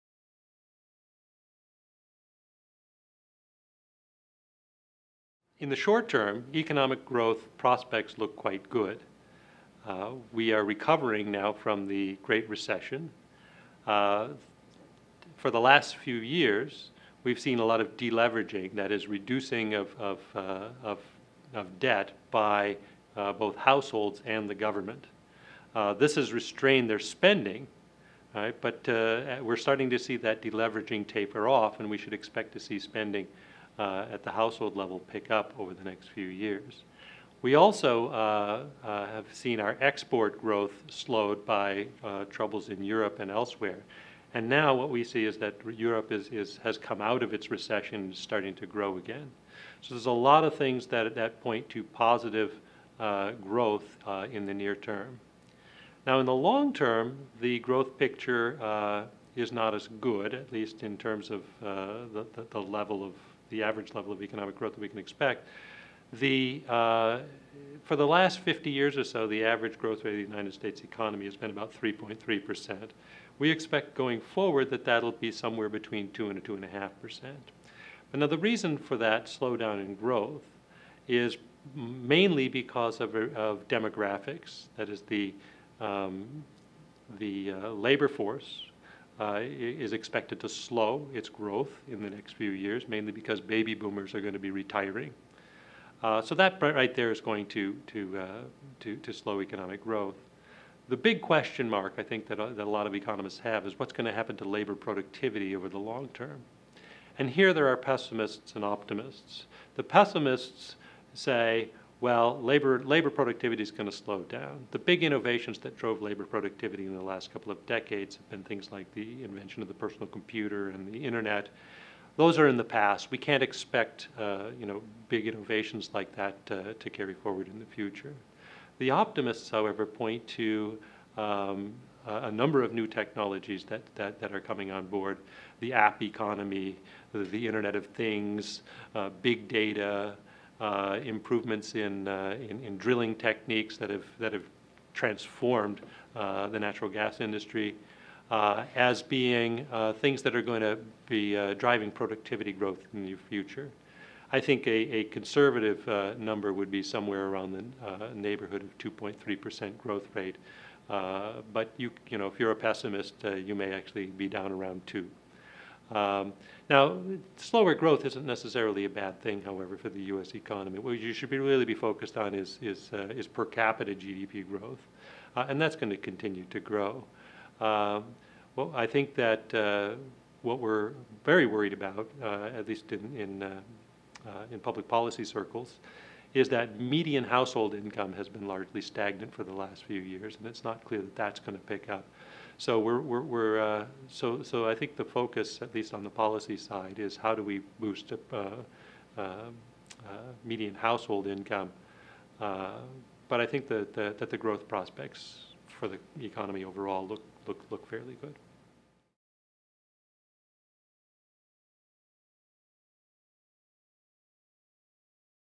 Short & Long-Term Economic Growth | Dr. Rod Ludema, Chief Economist, US Department of State
The Chief Economist for the US Department of State says our economy is growing in the short term, and household spending is increasing. But what will the long-term hold? There are challenges ahead, he says.